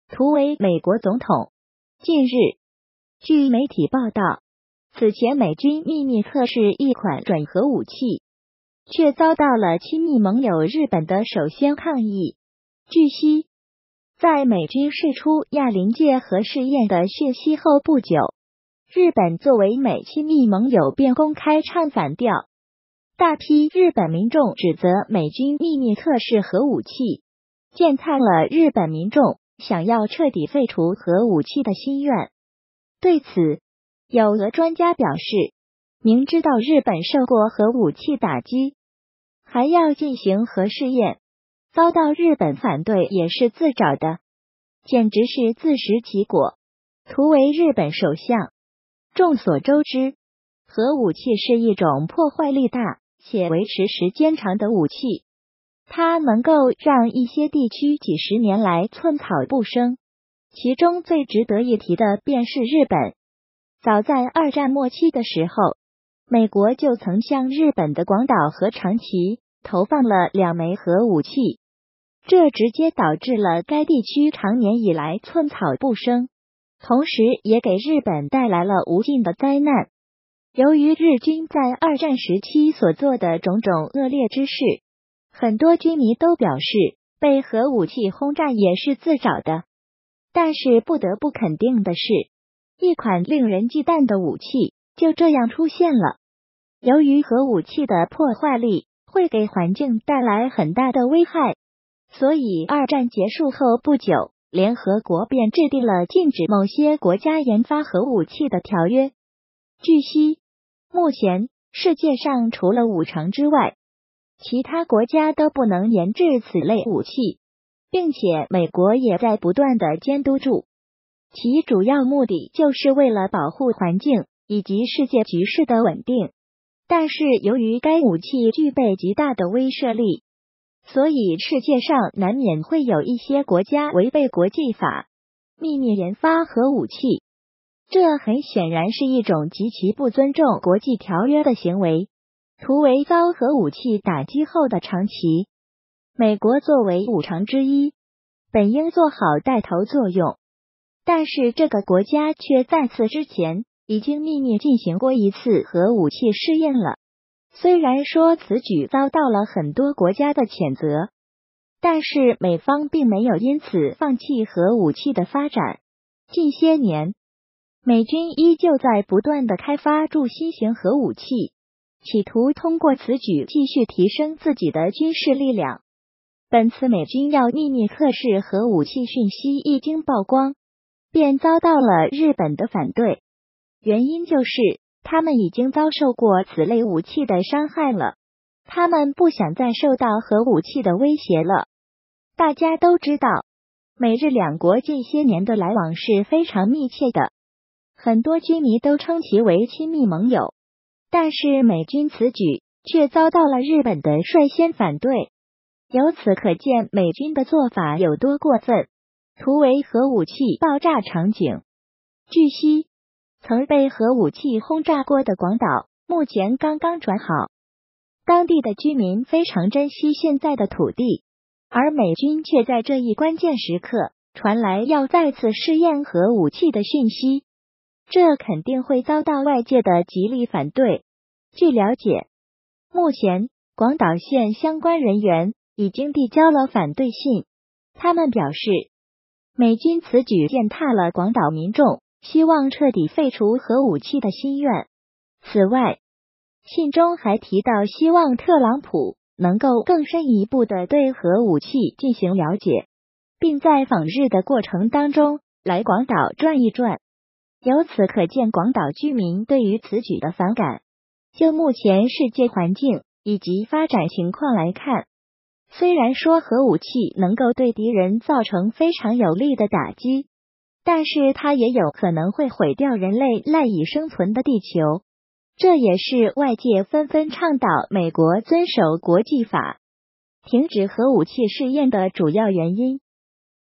美親密盟友公開唱反調，指責美軍祕密測試核武器，俄：自找的
圖為美國總統近日，此前美軍祕密測試一款“準核武器”，卻遭到了親密盟友日本的首先抗議，據悉，在美軍釋出亞臨界核試驗的訊息後不久。